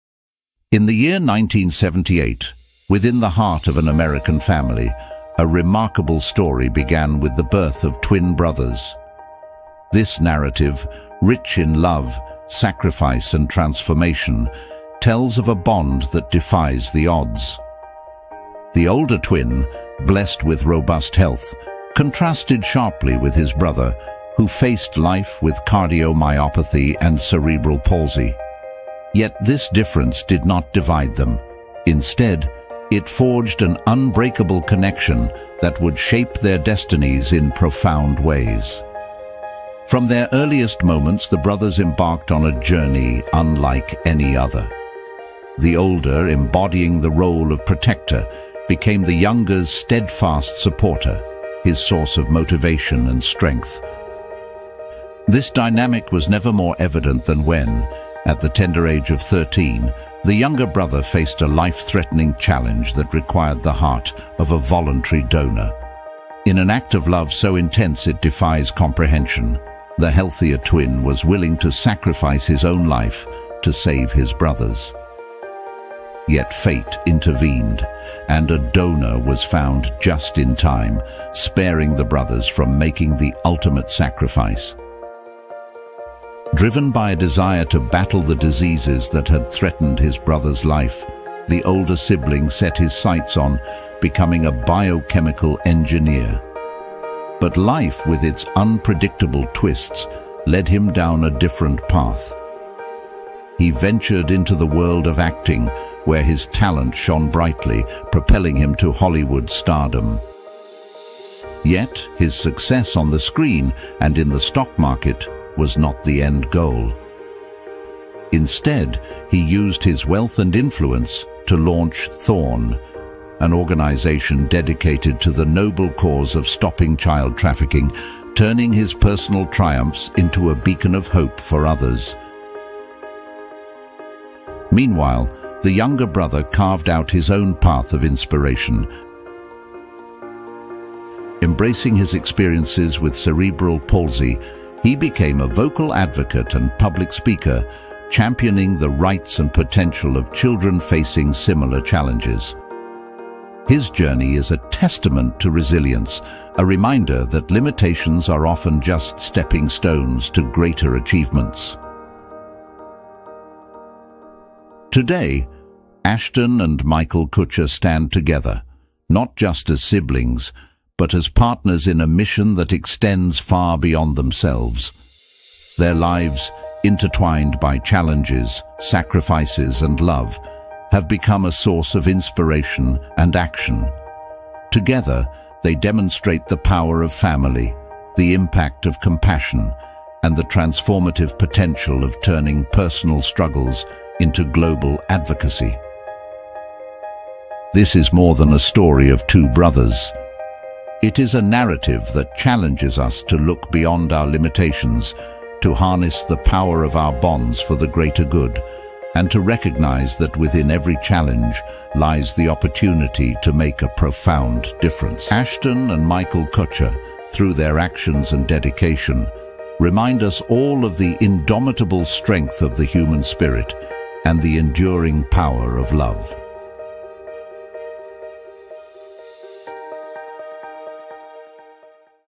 ASHTON KUTCHER: Here's WHY I'll SACRIFICE My Life For My TWIN
Dive into the heartwarming and inspiring journey of twin brothers Ashton and Michael Kutcher, whose lives have been defined by love, sacrifice, and an unwavering bond. Born into an American family in 1978, these brothers faced contrasting destinies from the start. While Ashton was blessed with health, Michael battled cardiomyopathy and cerebral palsy. This story unfolds the profound depths of brotherly love, showcasing how the brothers grew closer through adversity, with Ashton even willing to make the ultimate sacrifice to save Michael's life.

Discover how their paths diverged, with Ashton finding unexpected fame and success in Hollywood, only to use his platform and resources to combat child trafficking through his organization, THORN. Meanwhile, Michael became a powerful advocate for children with cerebral palsy, using his voice to inspire and effect change. Together, they exemplify how challenges can be transformed into opportunities to make a significant impact on the world.

Join us in exploring the remarkable lives of the Kutcher brothers, a testament to the power of family, the importance of giving back, and the unstoppable force of human compassion. Their story is not just inspiring; it's a call to action for all of us to make a difference in the lives of others.